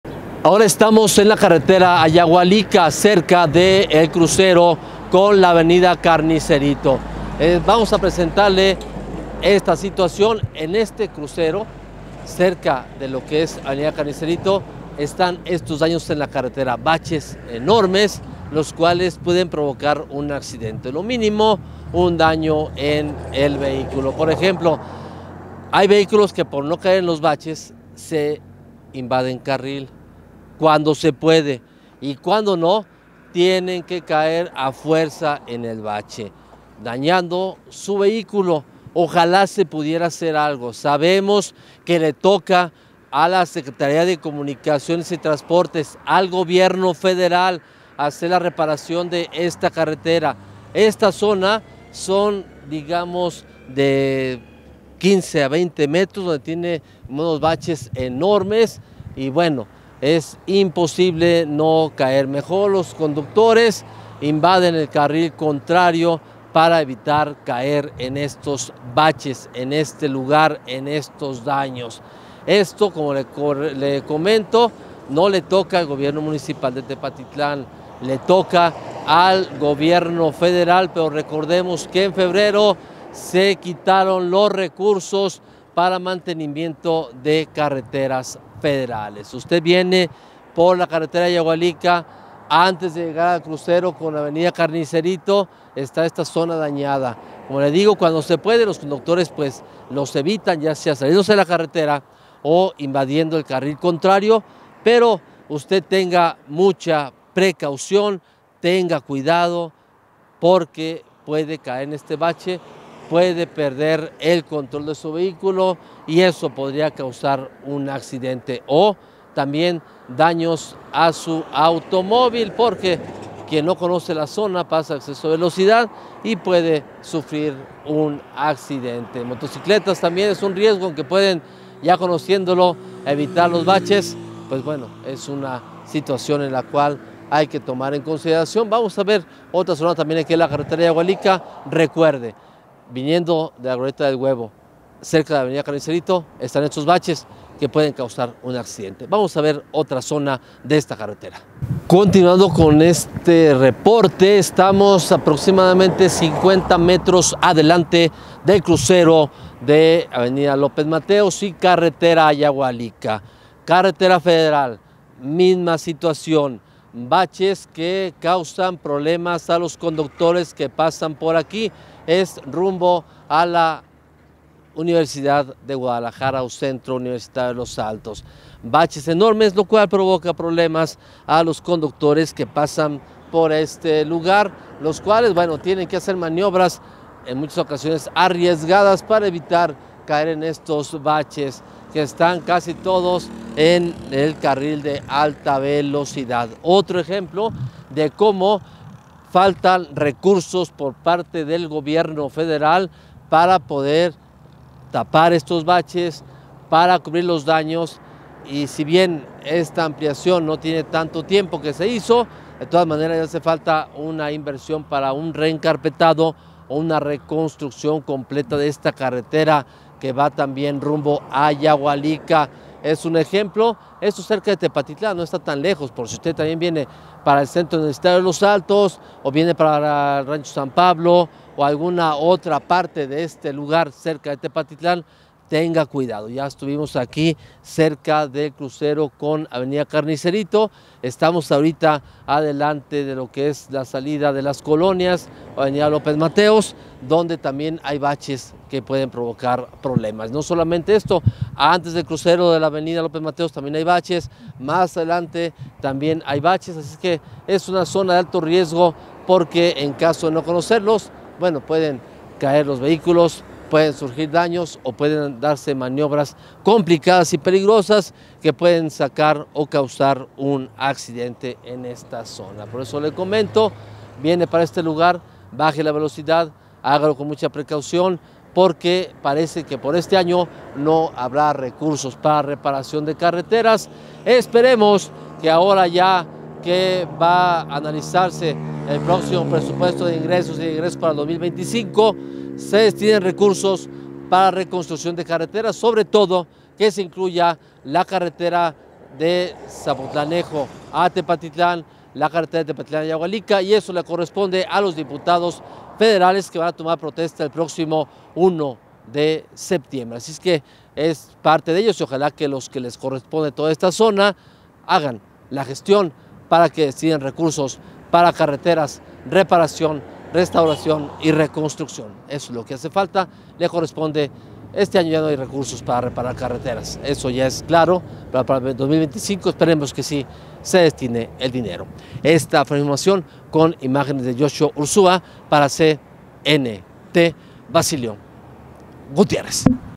Baches en la Carretera a Yahualica: Un Riesgo Constante
Exploramos la situación crítica de los baches en la carretera a Yahualica.  Estos baches representan un riesgo constante para los conductores .